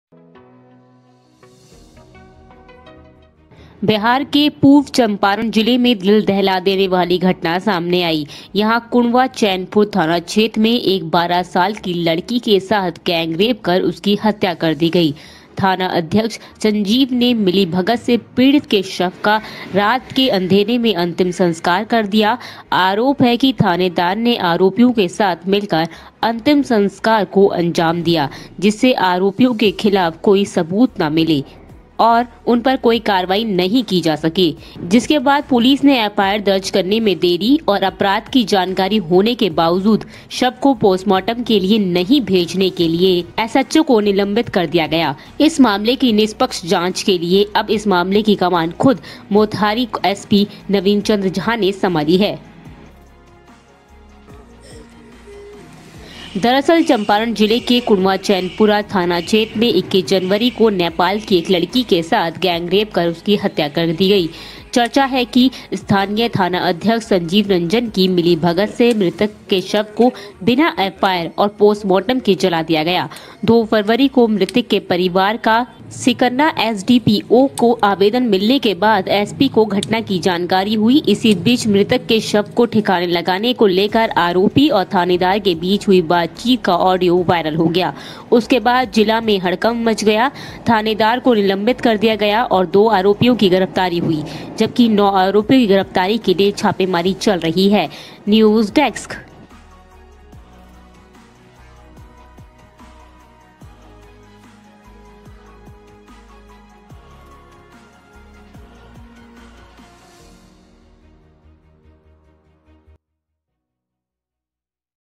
नाबालिग के साथ गैंगरेप के बाद हत्या, थानेदार ने सबूत मिटाने के लिए रात में किया अंतिम संस्कार किया
बिहार के पूर्वी चंपारण जिले में दिल दहला देने वाली घटना सामने आई हैं.. यहीं कुंडवा चैनपुर थाना क्षेत्र में एर 12 साल की लड़की के साथ गैंगरेप कर उसकी हत्या कर दी गई... थानाध्यक्ष संजीव ने मिलीभगत से पीड़ित के शव का रात के अंधेर में अंतिम संस्कार कर दिया... आरोप है कि थानेदार ने आरोपियों के साथ मिलकर अंतिम संस्कार को अंजाम दिया जिससे आरोपियों के खिलाफ कोई सबूत ना मिले जिससे कार्रवाई की जा सके... जिसके बाद पुलिस ने एफआईआर दर्ज करने में देरी और अपराध की जानकारी होने के बावजूद शव को पोस्टमार्टम के लिए नहीं भेजने के लिए एसएचओ को निलंबित कर दिया है... इस मामले की निष्पक्ष जांच के लिए अब इस मामले की कमान खुद मोतिहारी एसपी नवीन चंद्र झा ने संभाली है...